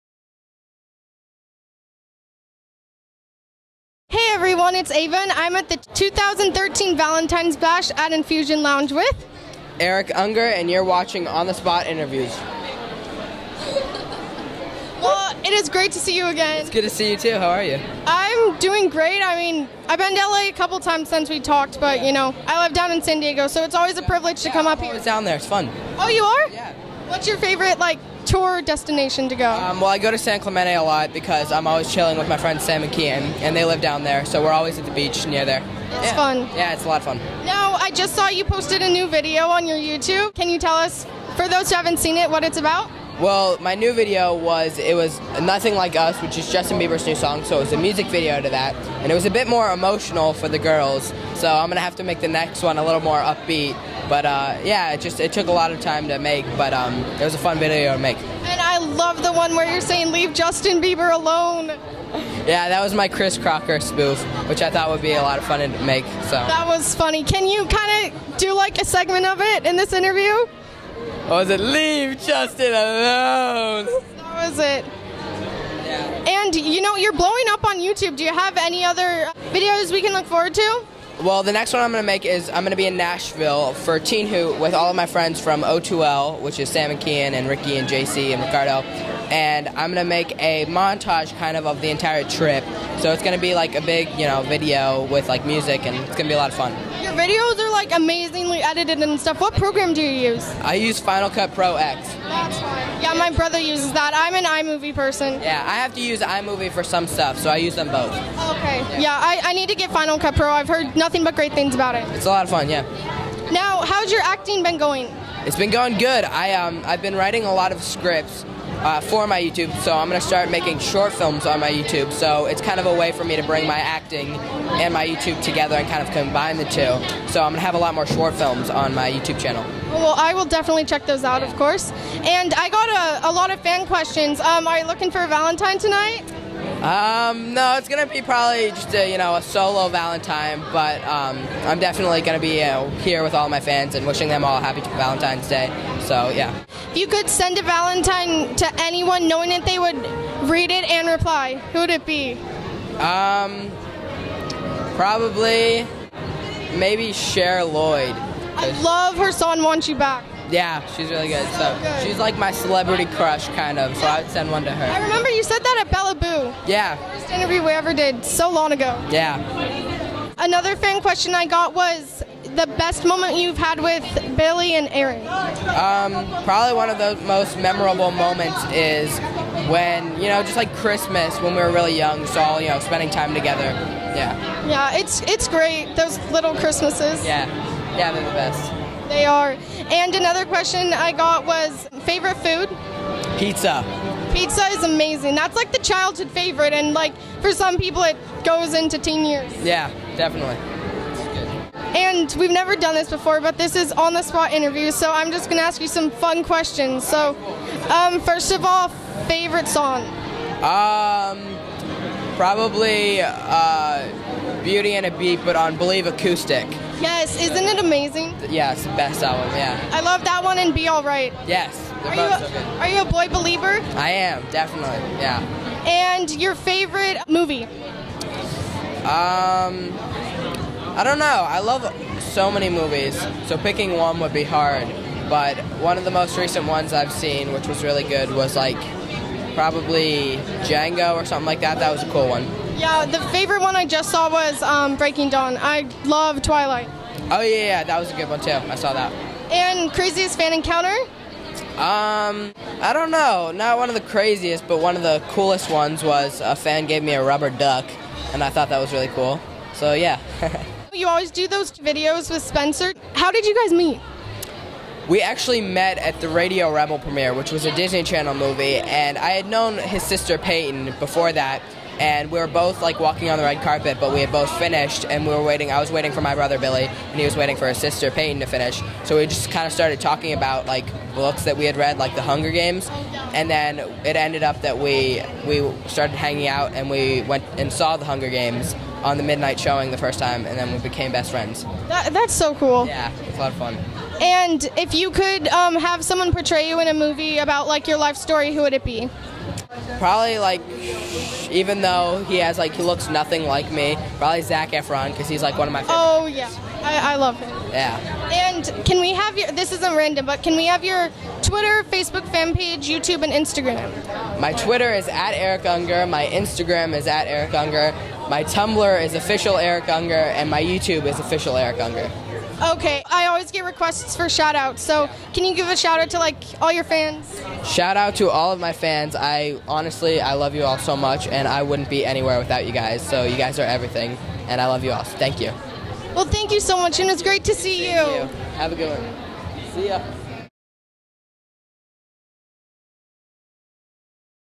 Eric Unger Interview at Valentines Bash 2013
What a great time Eric Unger and I had interviewing! He's always a blast to interview and I think you'll all really enjoy this ones. We included some fan questions as well as his youtube videos and acting.